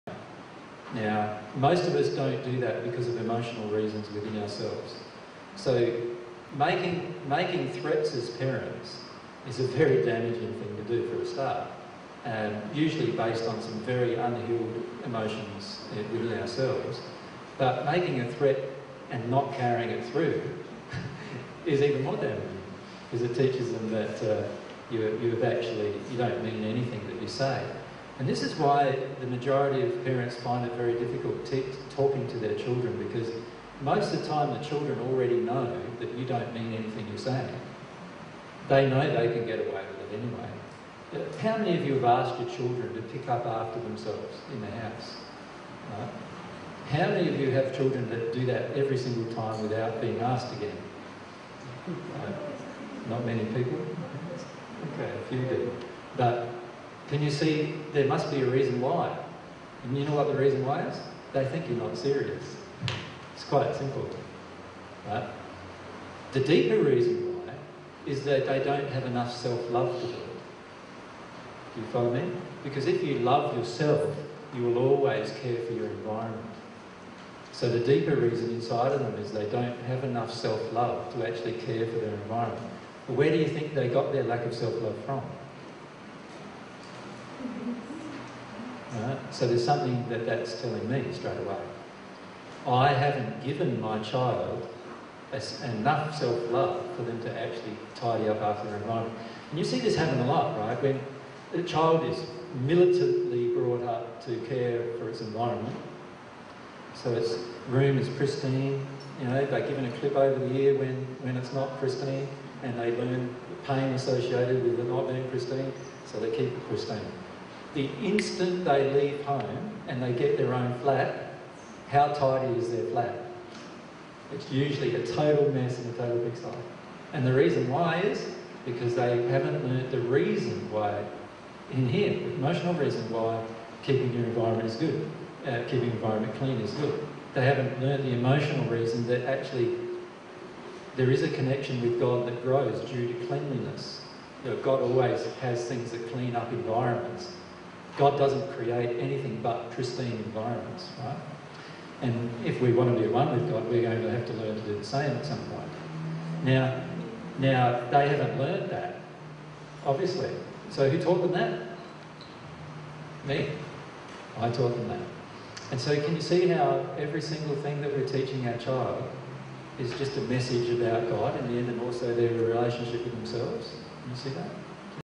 Jesus on Children, Cleanliness and God
Jesus discusses teaching children about cleaning up after themselves, caring for their environment and God's truth on the matter.

This is a section of the presentation by Jesus entitled "20090307 Human Relationships - Parenting Children S1P1".

dtstart:010444:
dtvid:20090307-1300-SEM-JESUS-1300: